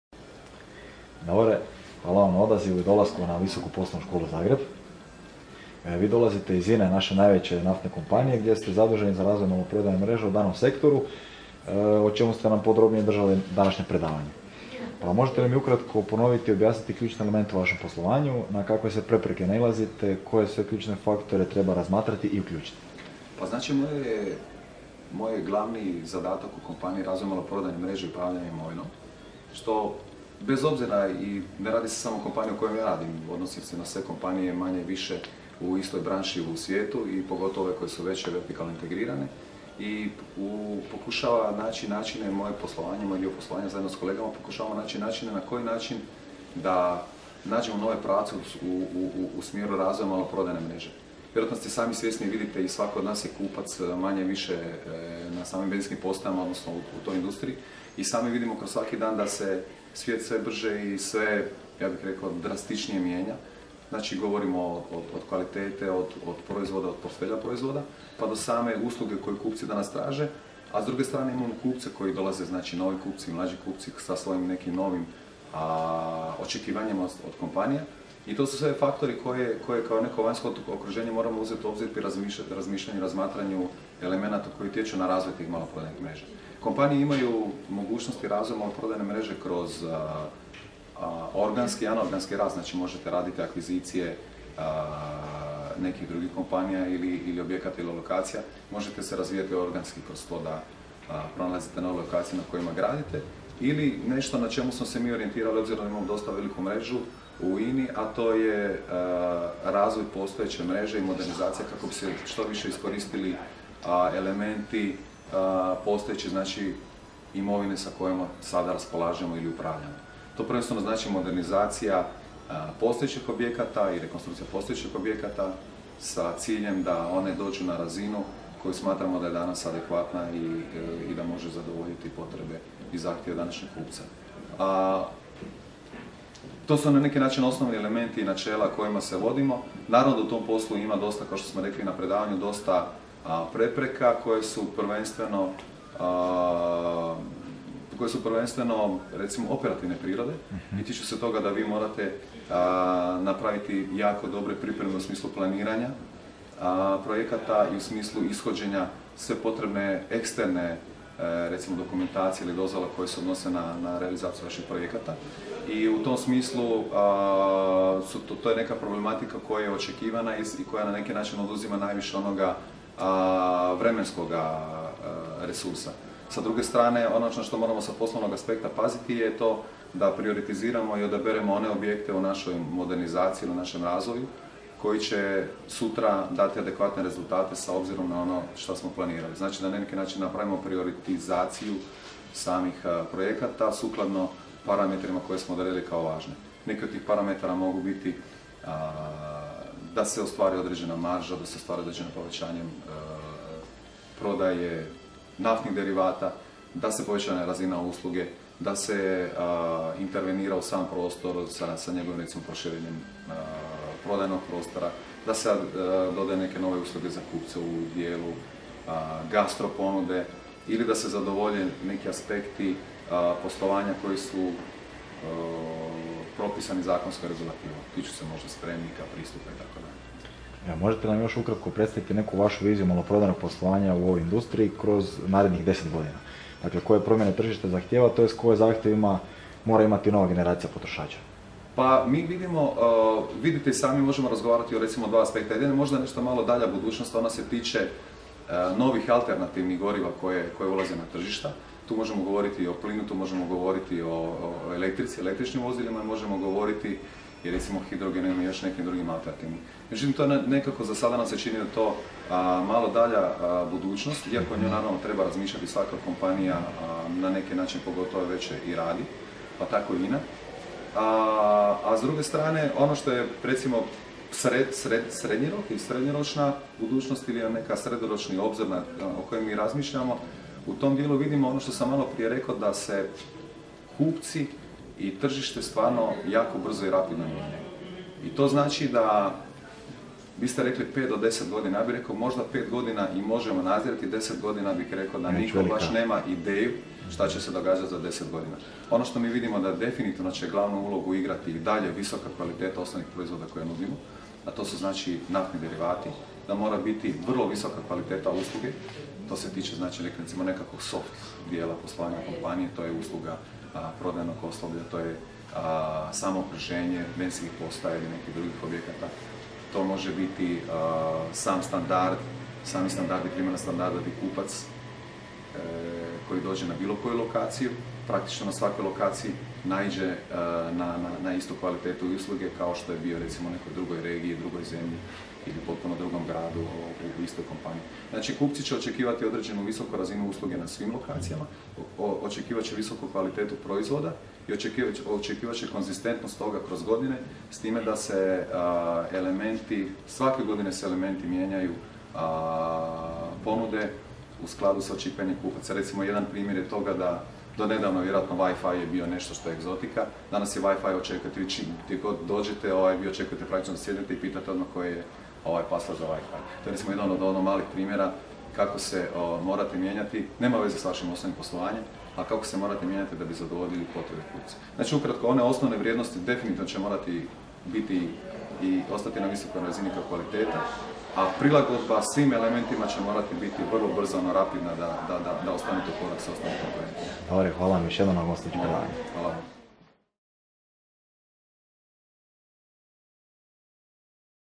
Davor Knez - Intervju VPŠZ
Intervju povodom gostujućeg predavanja na Visokoj poslovnoj školi Zagreb